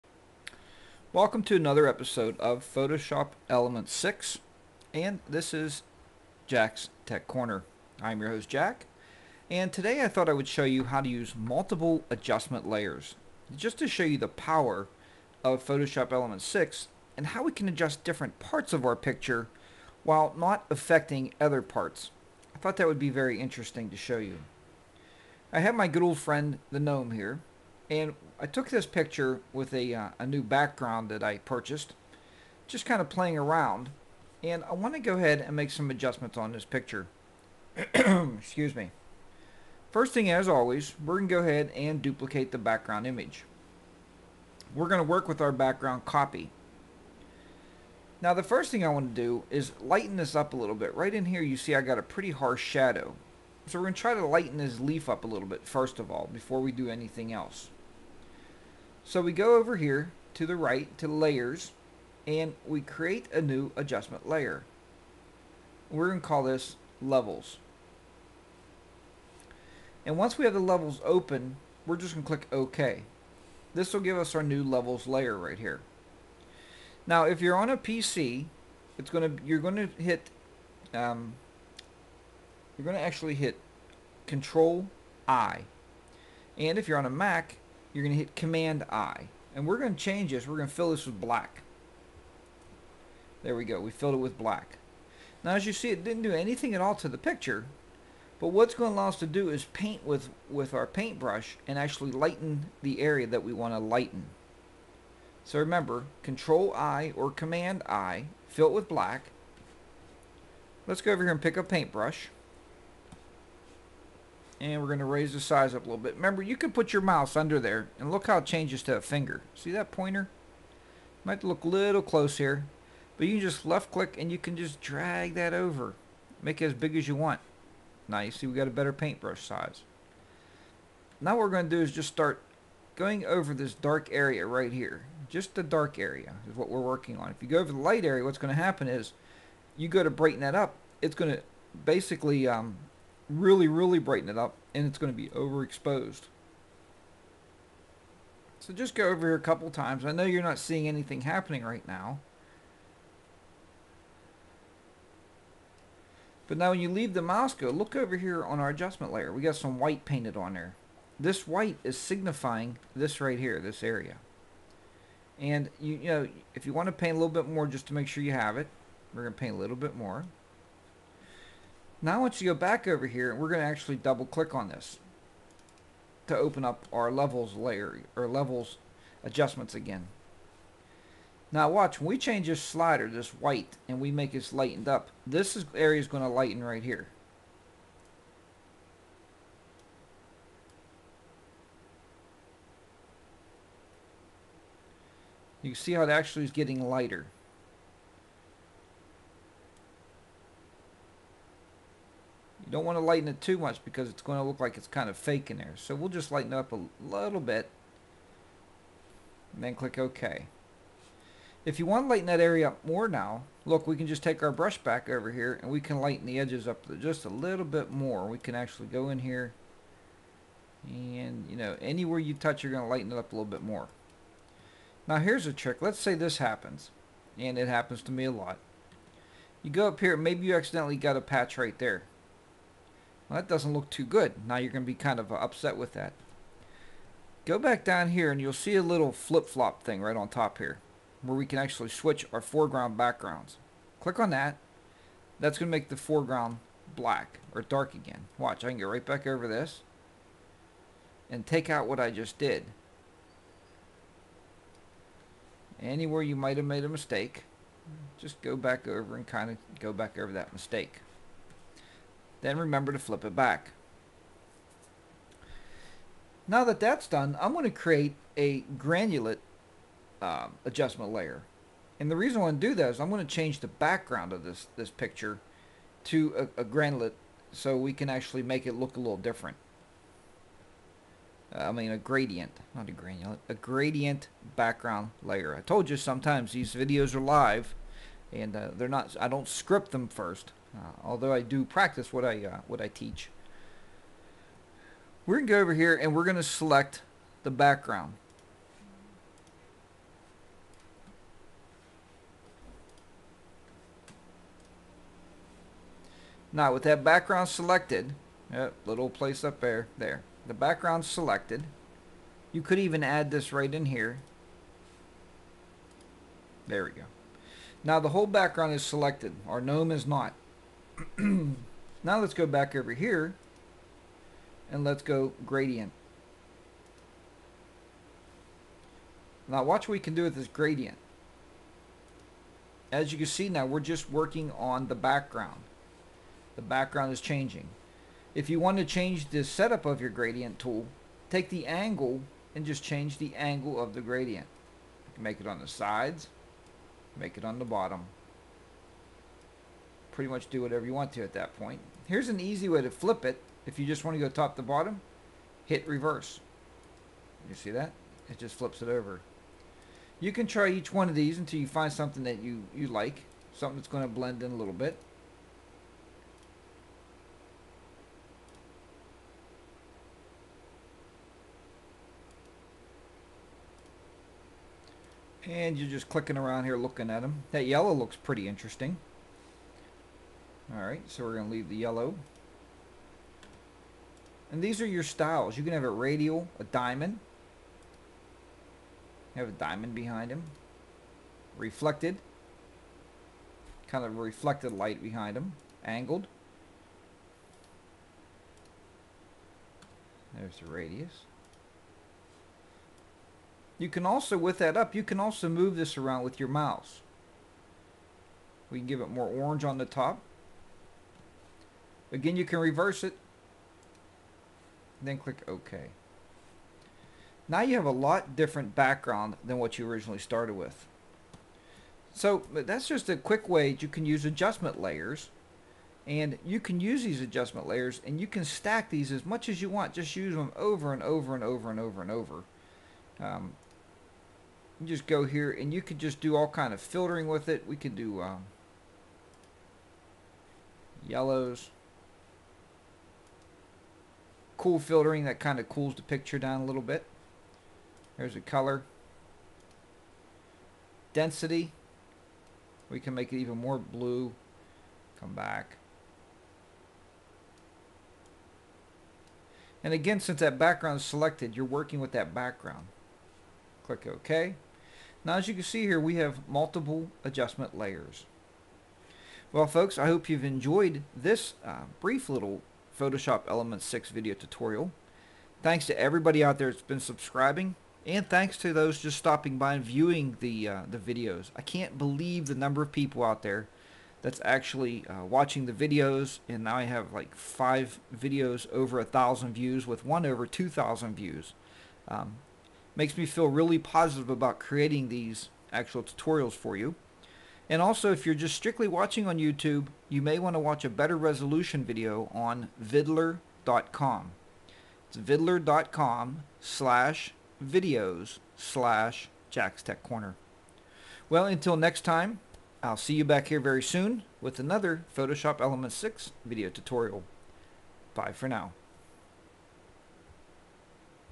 Using Multiple Adjustment Layers
In this video we will see how we can use multiple adjustment layers. This will give us greater control on fine tuning our pictures.